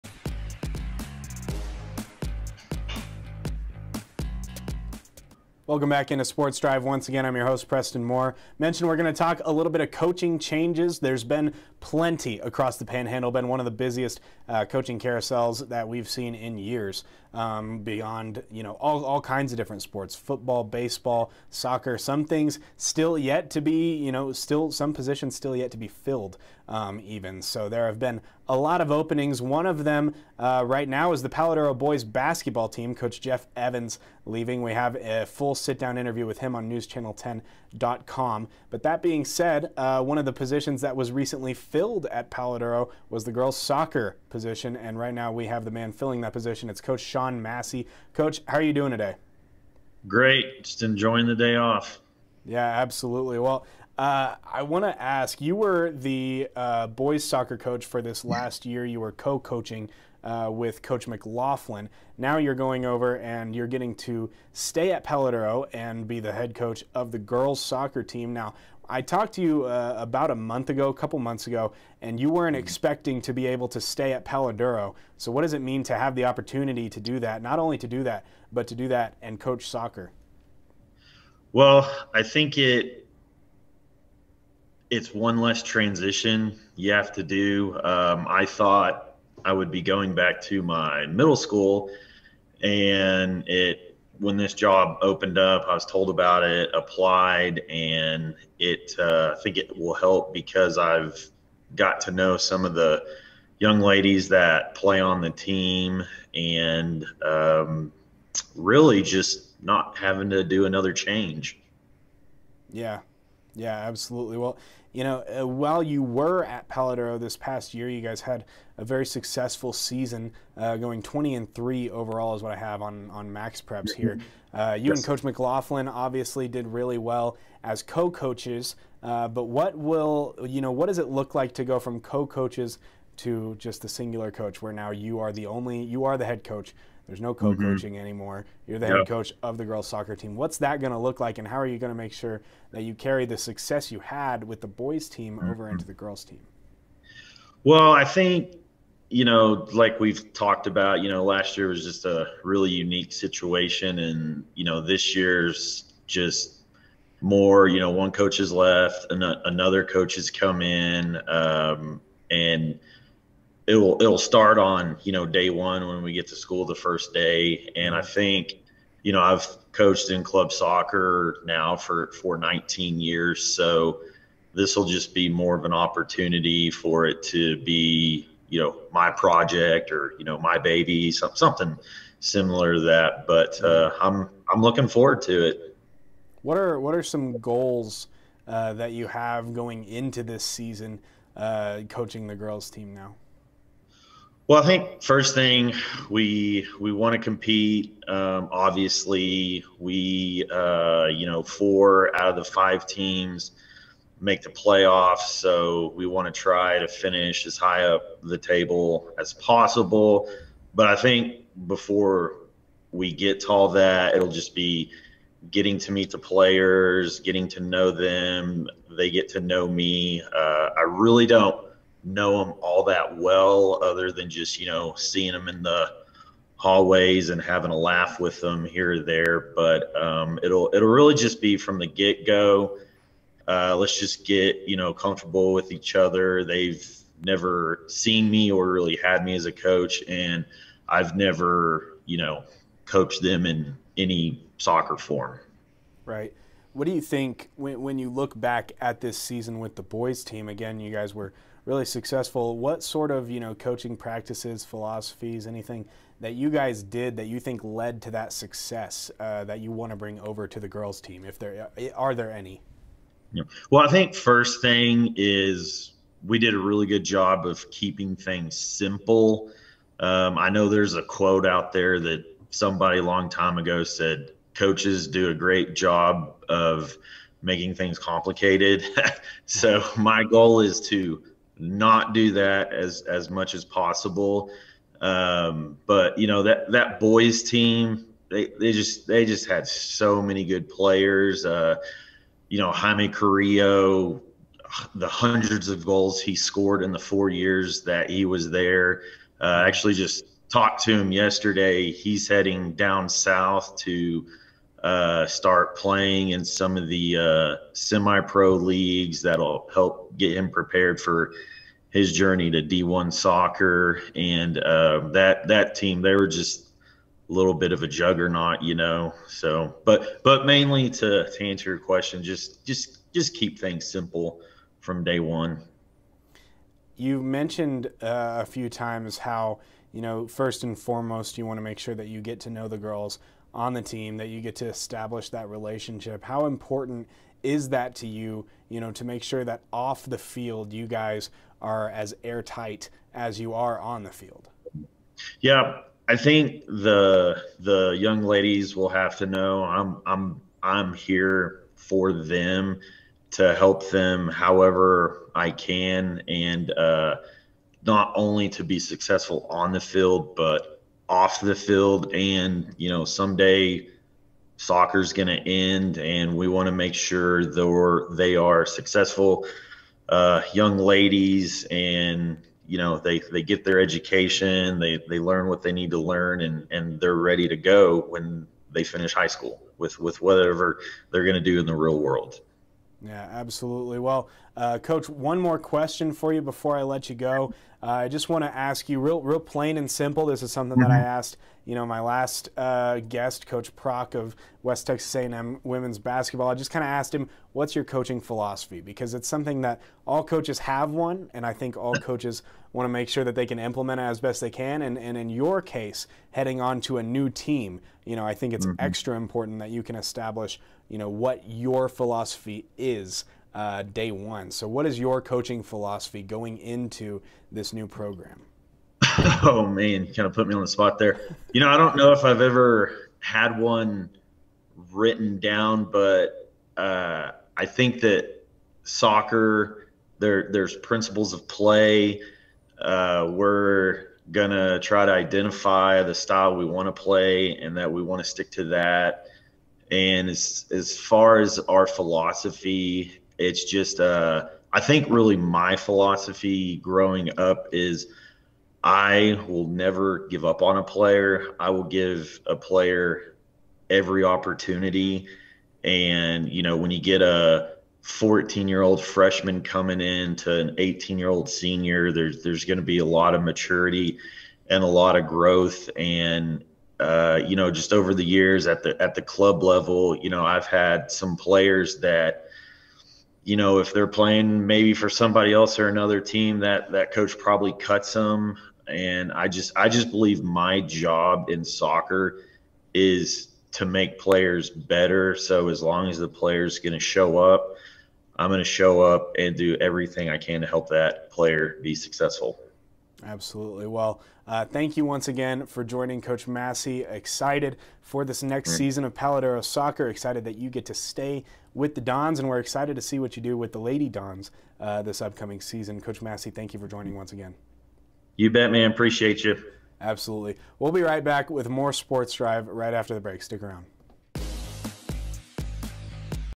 SPORTS DRIVE: Coach Massie tells us about becoming head coach for the Palo Duro girl's soccer tea...
SPORTS DRIVE: Coach Massie tells us about becoming head coach for the Palo Duro girl's soccer team!